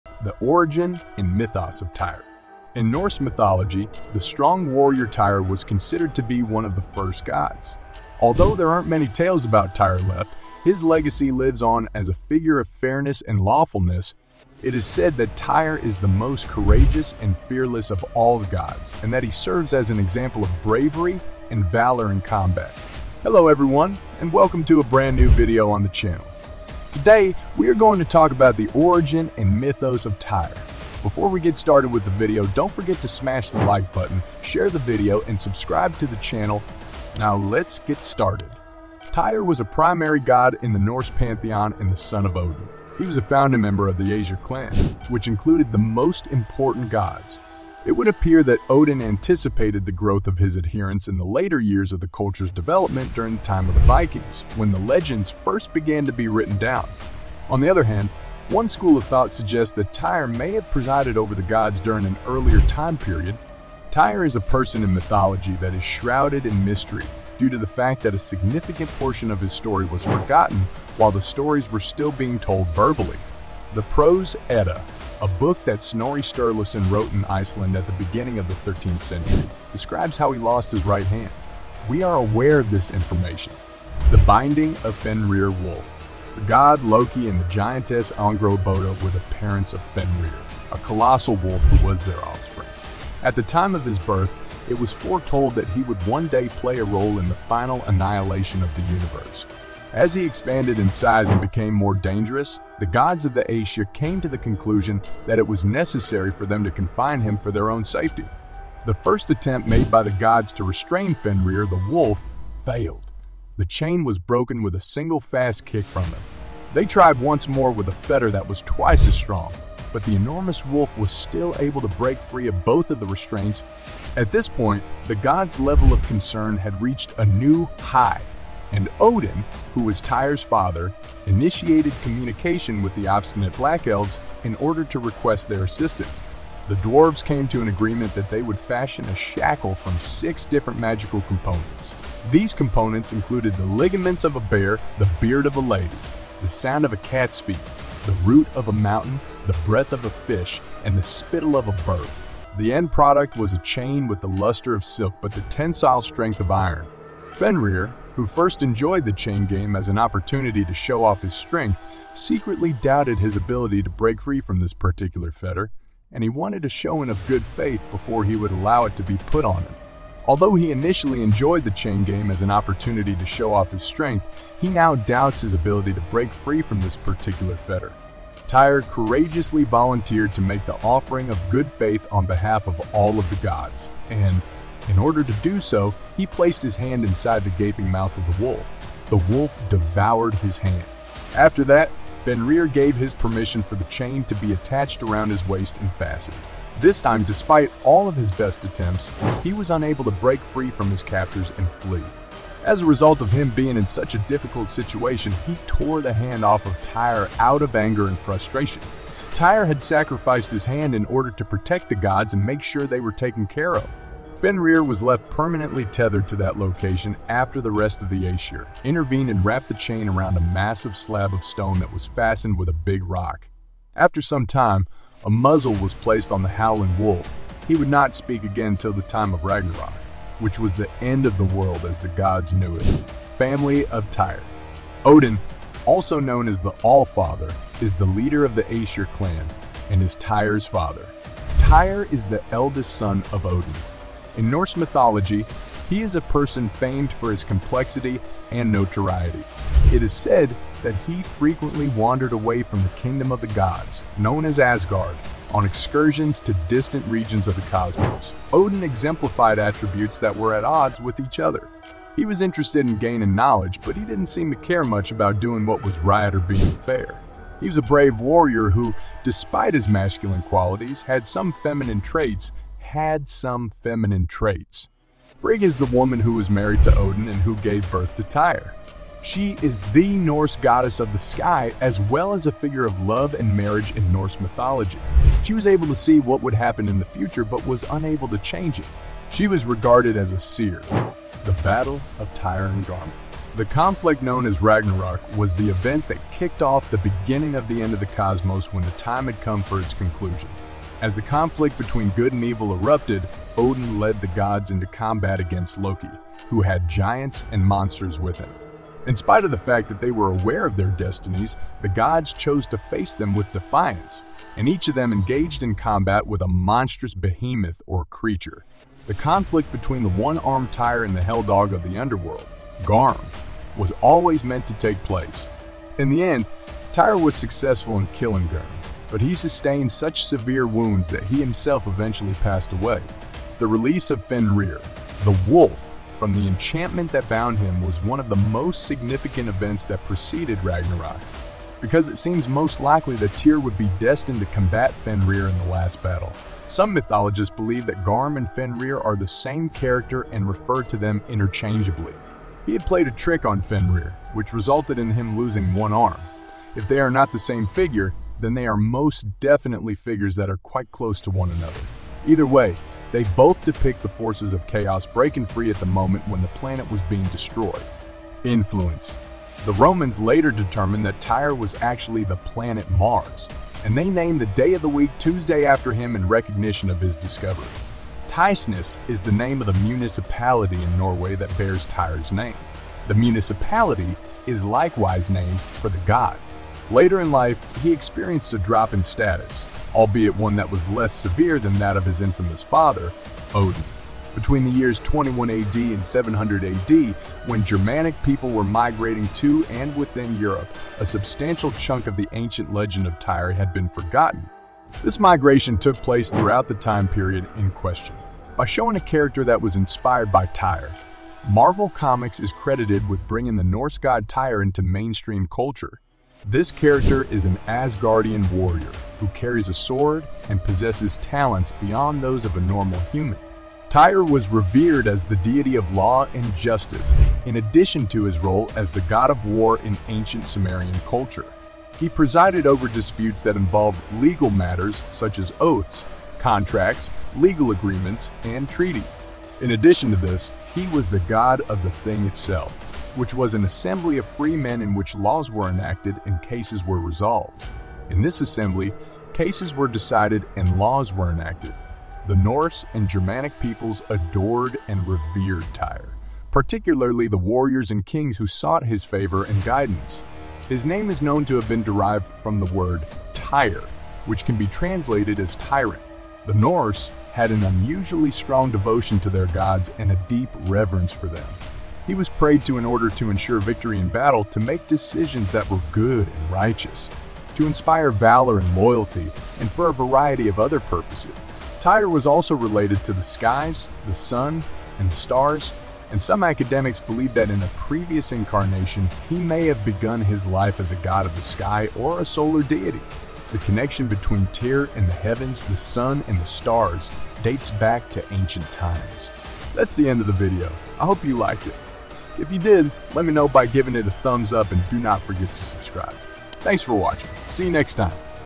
Unveiling the Mythos and Origins of Tyr - Norse God of War
Title: "Unveiling the Mythos and Origins of Tyr - Norse God of War"

Description:
In this episode, we're diving headfirst into the captivating world of Norse mythology to uncover the mythos and origins of Tyr, the fearless god of war.

Tyr is a formidable figure in the Norse pantheon, known for his courage, unwavering honor, and dedication to justice. Join us as we explore the fascinating stories and historical context surrounding this iconic deity.

In this video, you can expect to:

🛡️ Discover the role of Tyr in the Norse pantheon and his connection to other gods and goddesses.
⚔️ Unearth the legendary tale of how Tyr lost his hand, an act of incredible sacrifice.
🌍 Explore the historical and cultural background of the Norse people and how it influenced the stories of Tyr.
🔮 Gain insights into the enduring legacy of Tyr in modern culture, from literature to pop culture references.

If you're a fan of mythology, history, or just curious about the origins of iconic figures like Tyr, be sure to hit that subscribe button and ring the notification bell, so you never miss an episode of our mythological journey.

Join us on this epic adventure as we unveil the mythos and origins of Tyr, the Norse god of war. 🌌🗡️